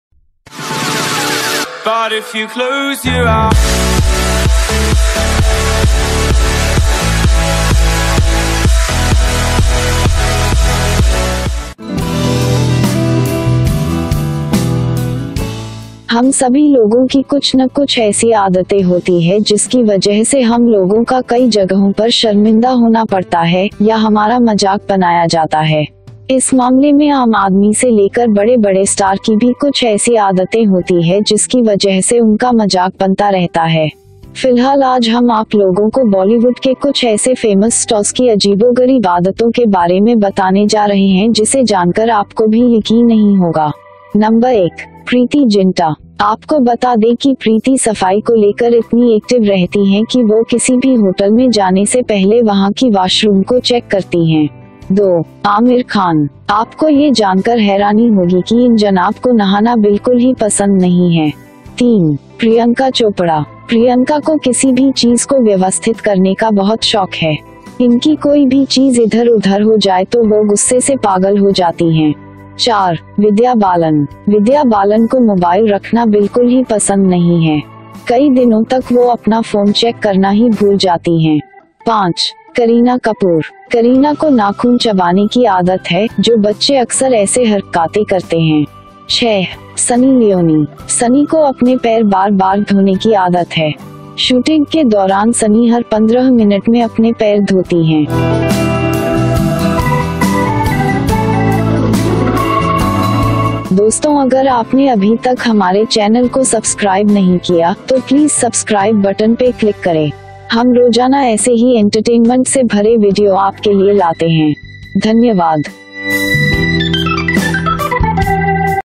बॉलीवुड स्टार्स की इन गंदी हरकतों को जानकर इनके बारे में सोचने पर मजबूर हो जायेंगे आप
हम सभी लोगों की कुछ न कुछ ऐसी आदते होती है जिसकी वजह से हम लोगों का कई जगहों पर शर्मिंदा होना पड़ता है या हमारा मजाक बनाया जाता है इस मामले में आम आदमी से लेकर बड़े बड़े स्टार की भी कुछ ऐसी आदतें होती है जिसकी वज्जः से उनका मजाक बनता रहता है.